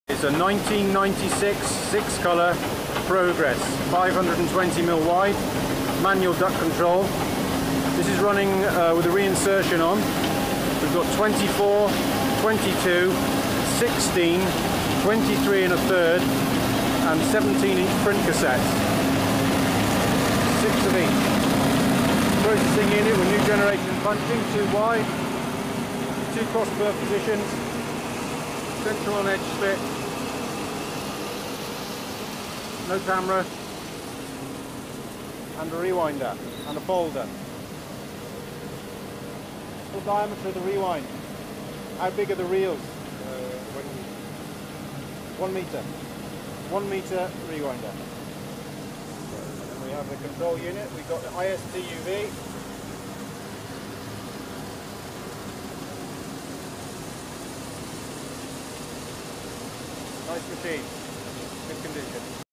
Muller Martini Progress 6 colour
Muller Martini Grapha Progres 520 mm web width 6 colour. reel toreel and reel to fold.